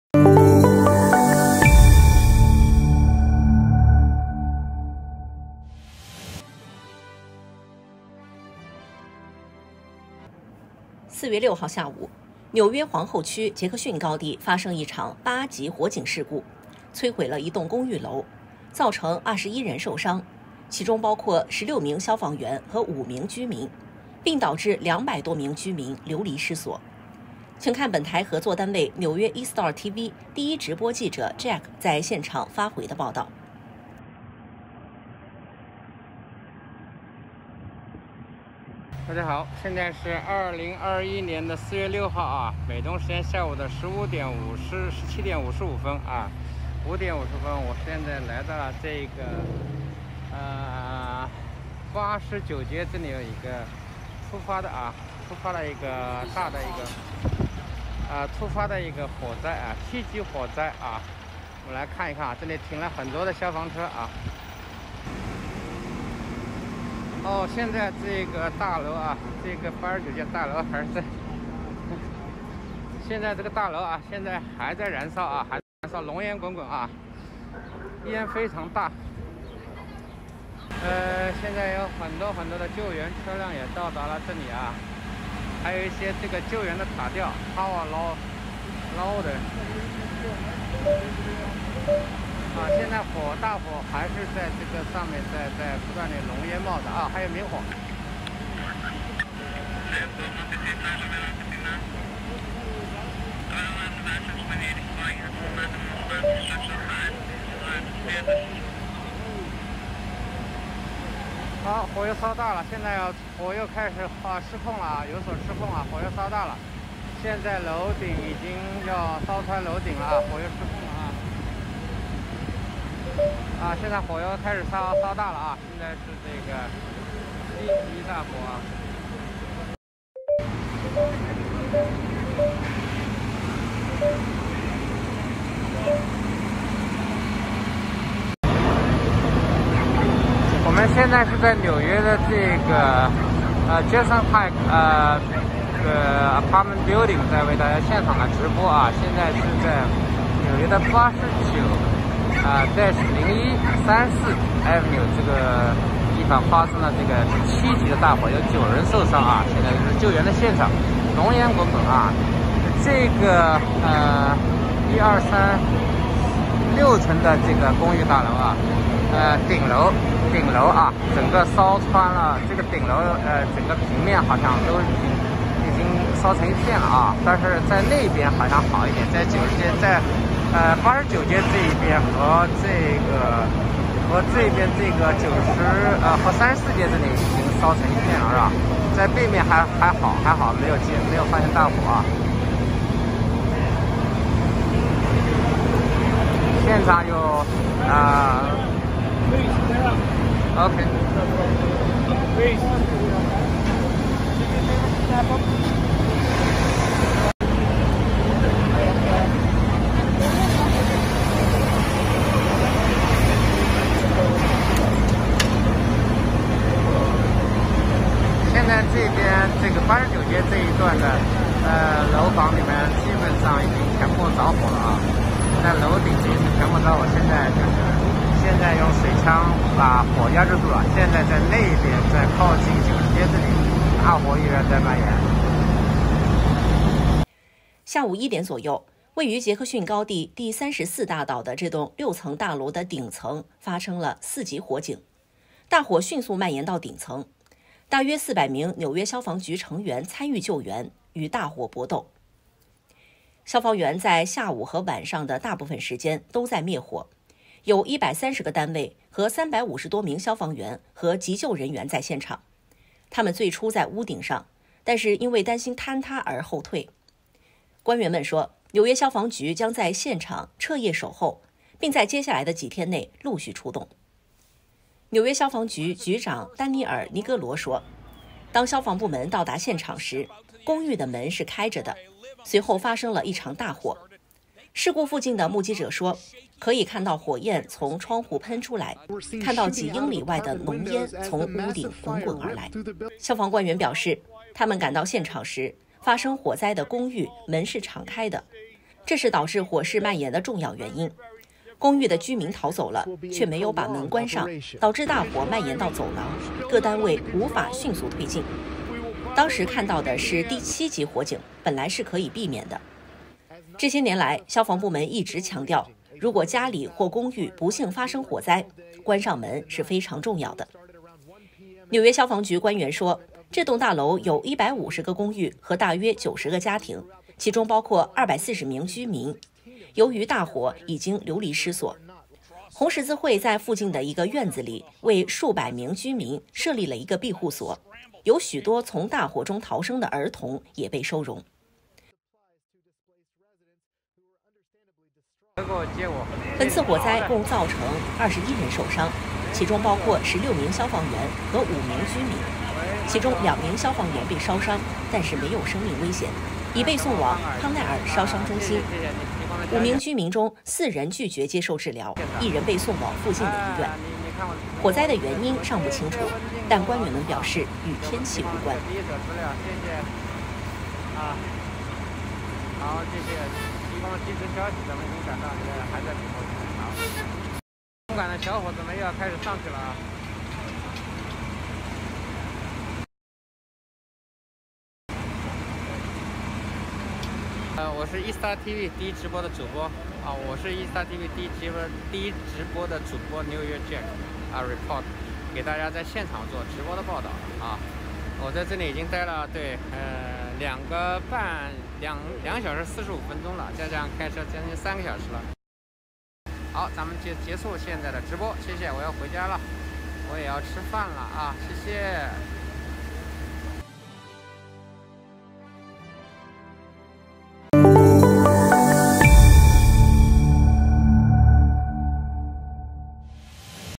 21人受伤，其中包括16名消防员5名居民，纽约纽约皇后公寓大楼发生8级火灾
纽约市皇后区杰克逊高地周二下午，一场八级火警摧毁了皇后区的一栋公寓楼，造成21人受伤，其中包括16名消防员和5名居民，并导致200多名居民90多个家庭流离失所。